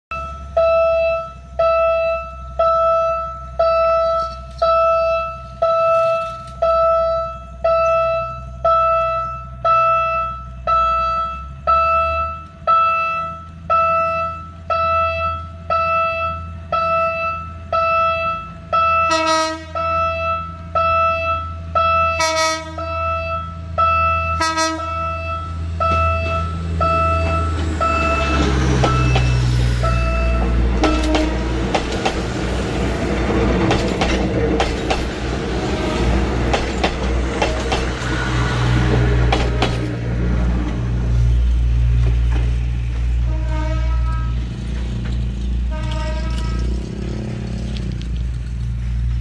PNR Inka DMU 8100 Class Set 2 @ PNR Sta. Mesa. First trip soutbound
train spotting at 530am for first trip